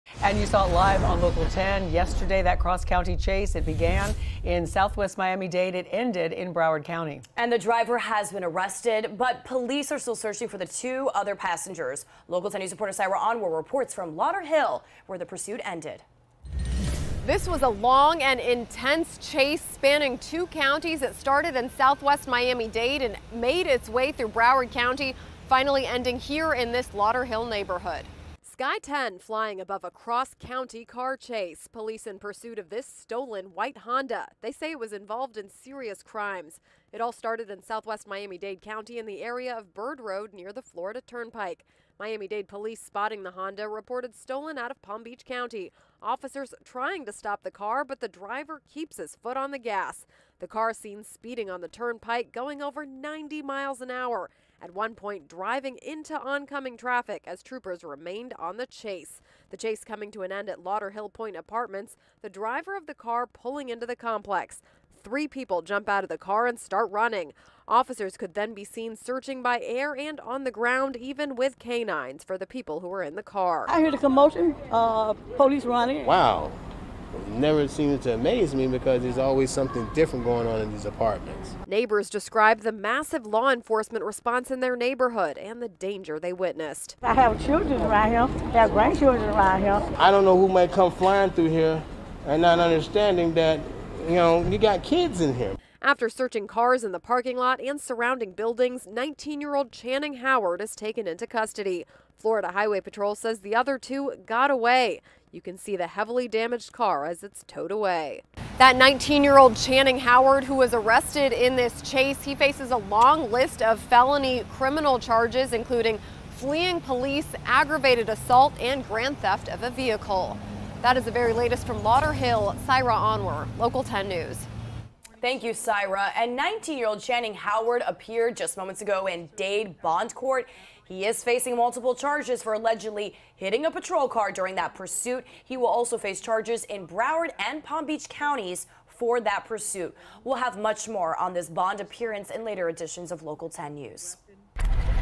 Pursuit suspect appears in Miami-Dade court
One of the men arrested following a cross-county chase that ended in Lauderhill Thursday appeared in Miami-Dade bond court Friday.